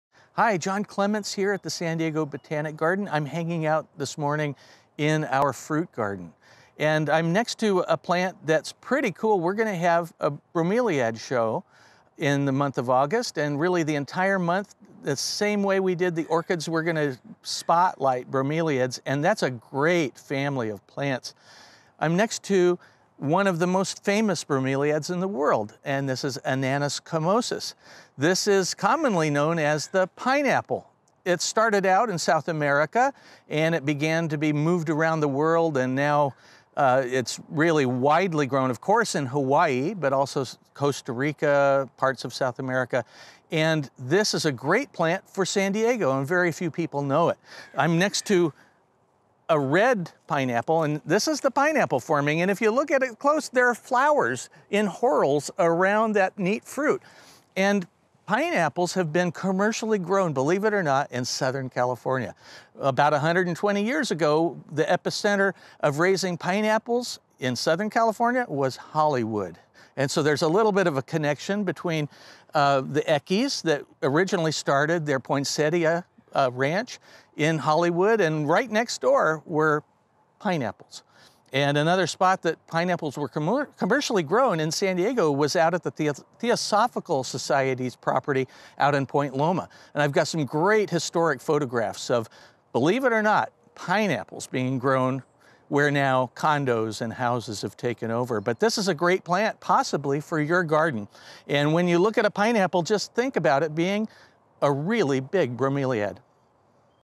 John's Cool Blooms & Interesting Plants, ananas comosus
John is joining us today from our fruit garden! He’s teaching us about one of the most famous bromeliads, 𝘈𝘯𝘢𝘯𝘢𝘴 𝘤𝘰𝘴𝘮𝘰𝘴𝘶𝘴, or the pineapple! 🍍 Learn about this DELICIOUS plant and start making your plans to joining us for our bromeliad show!